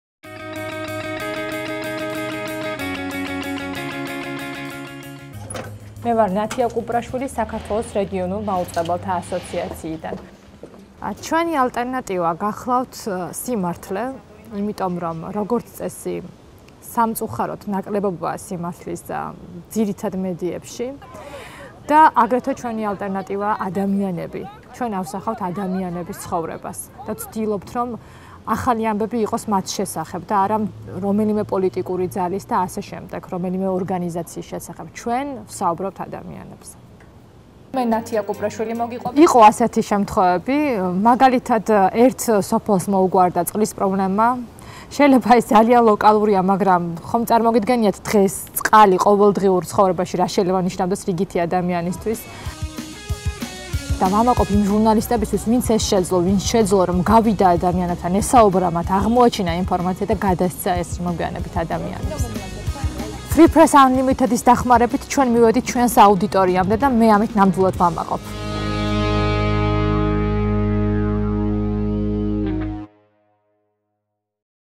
Regional news for everyone in Georgia
Natia Kuprashvili of the Georgian Association of Regional Broadcasters makes sure she brings reliable news that concerns all Georgians. With the support of Free Press Unlimited!